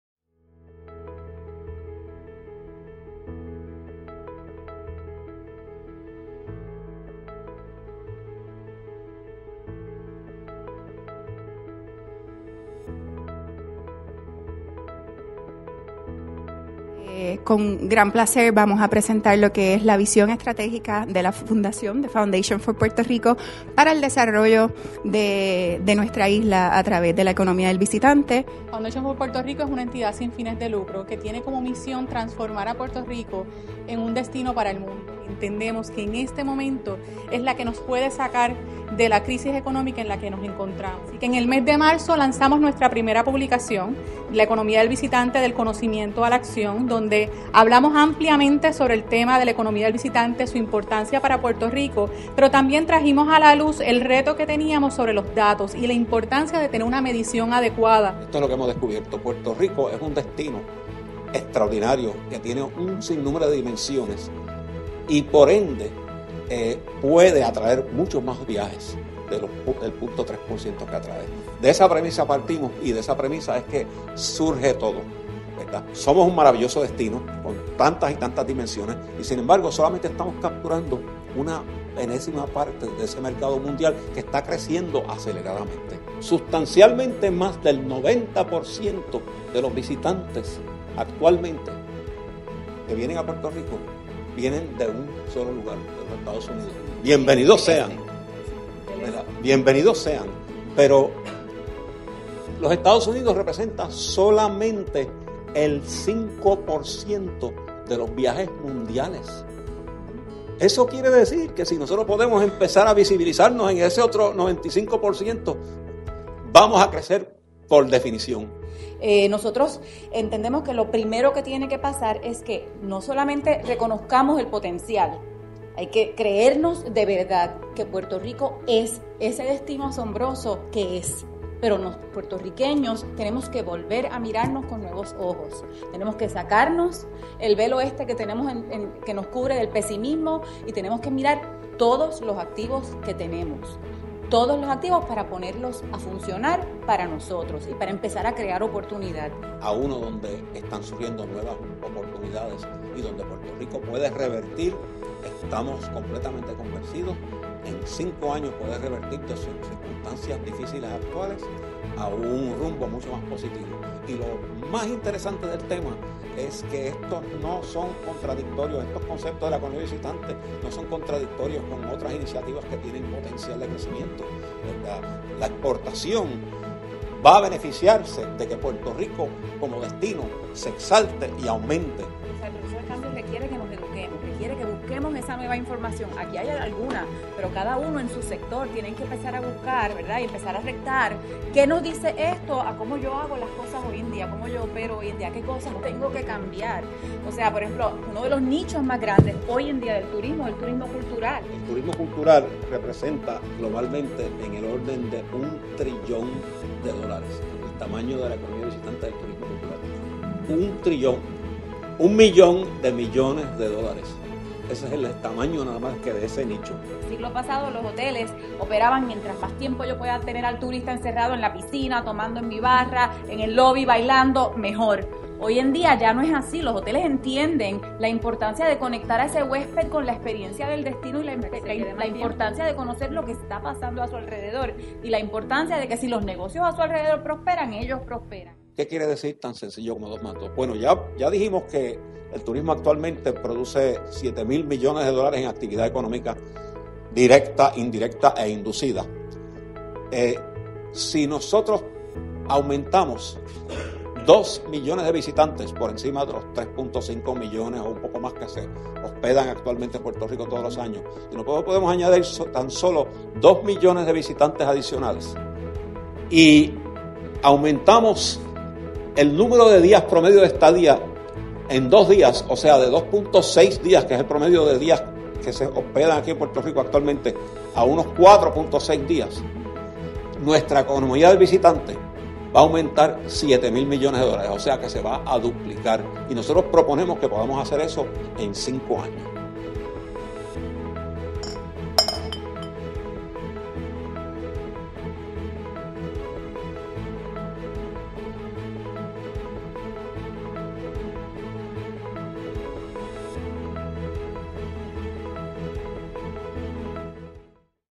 Conferencia de Prensa Visión Estratégica de Foundation for Puerto Rico
Presentación de la visión estratégica de Foundation for Puerto Rico para el desarrollo económico de Puerto Rico a través de la economía del visitante. ¡Tan sencillo como 2+2!